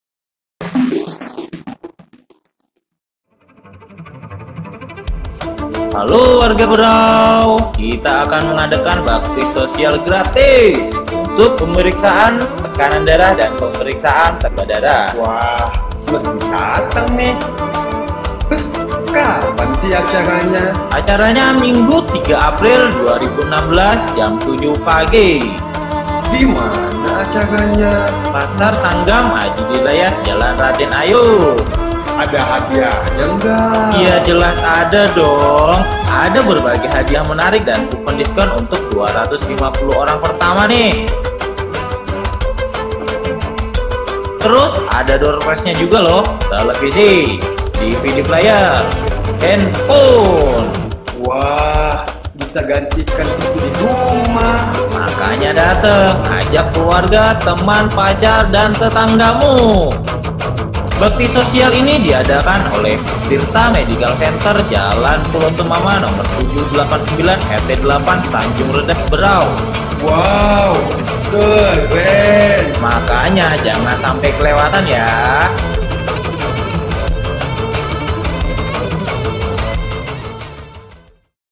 TIRTA MEDICAL CENTRE BAKSI SOSIAL
3 APRIL 2016
GRATIS PEMERIKSAAN TEKANAN DARAH
GRATIS PEMERIKSAAN GULA DARAH